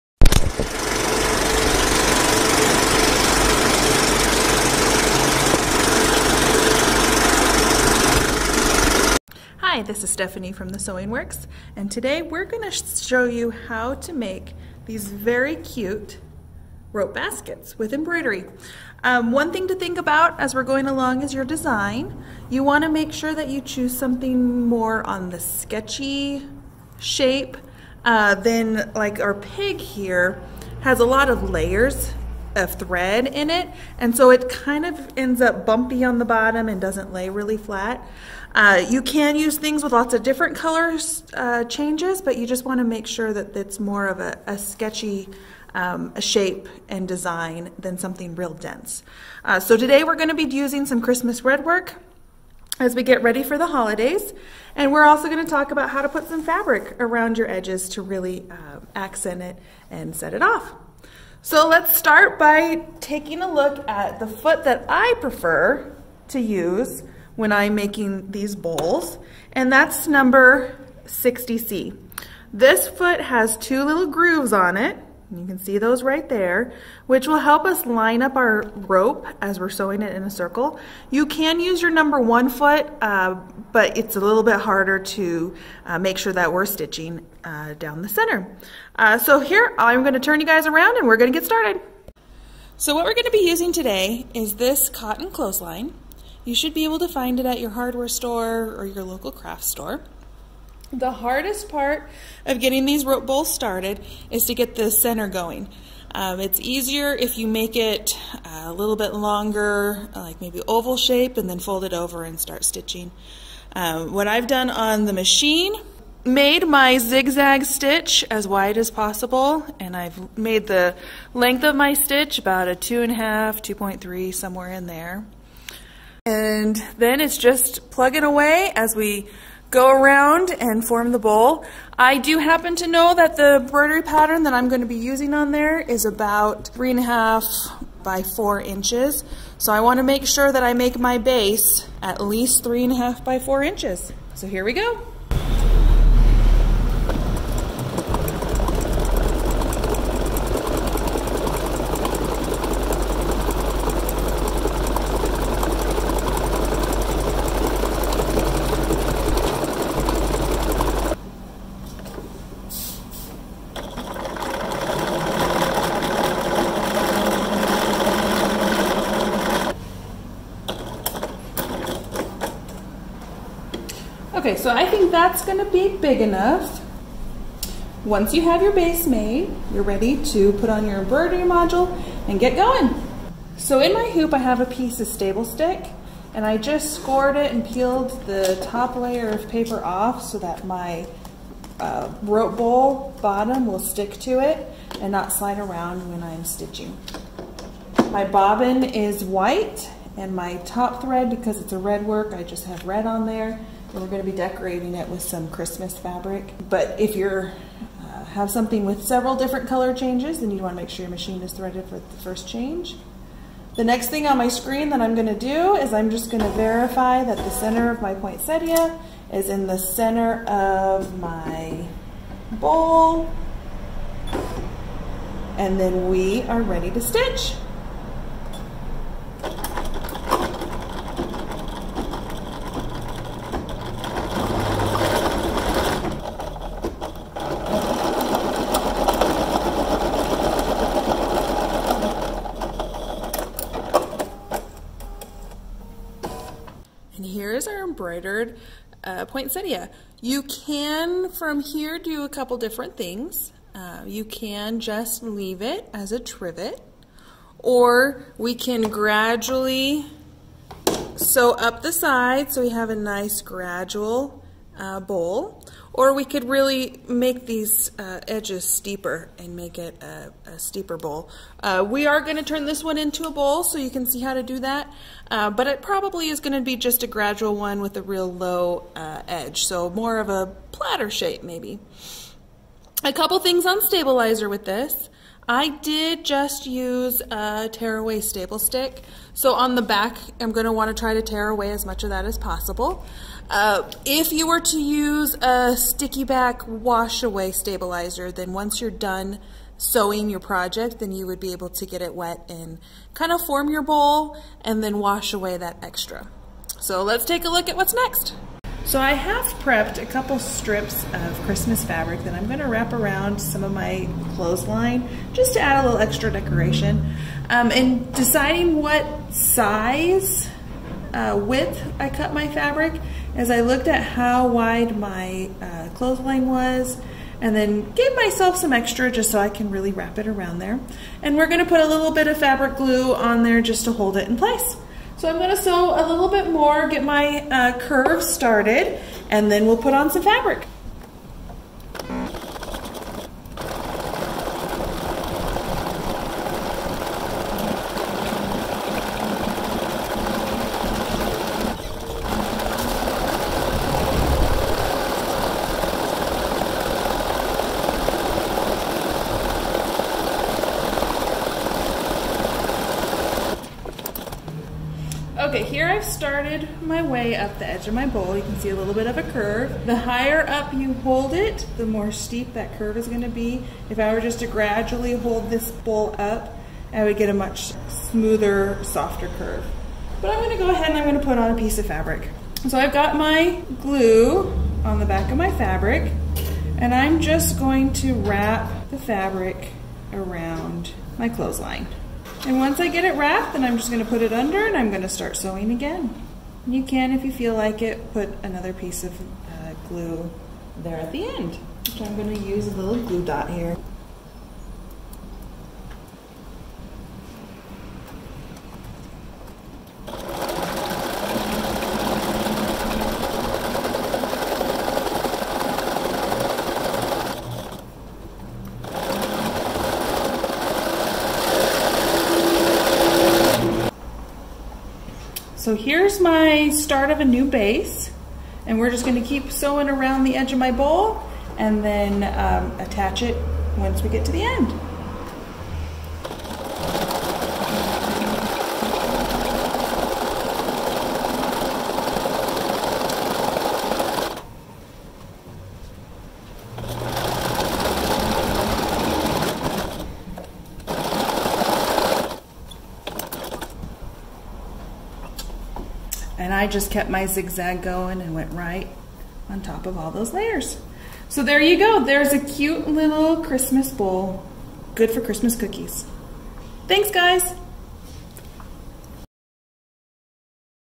Embroidered Rope Bowls
Stephanie of The Sewing Works shows us how to make rope bowls with embroidery on your sewing machine.